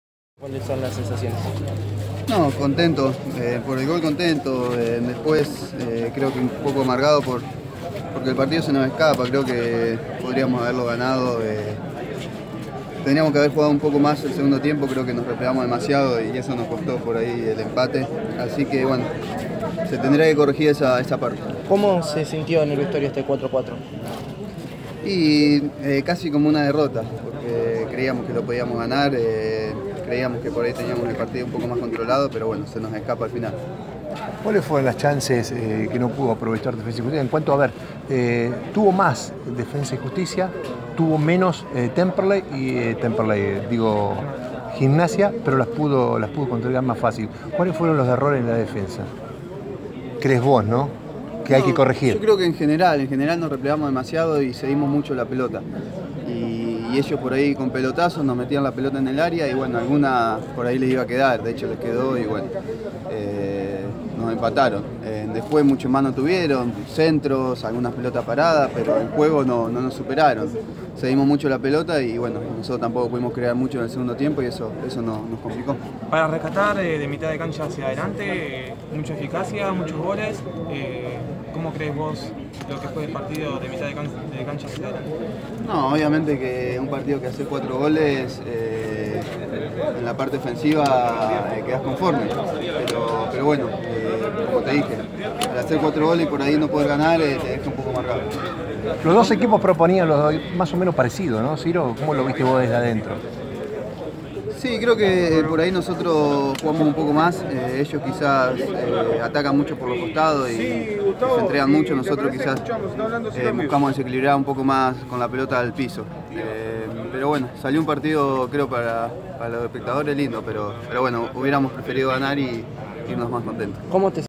Defensa 4-4 Gimnasia: La palabra de Rius
En Florencio Varela, el local y el "Tripero" igualaron en un entretenido encuentro. El delantero local, Ciro Rius dialogó con la prensa y detalló su mirada sobre el resultado final.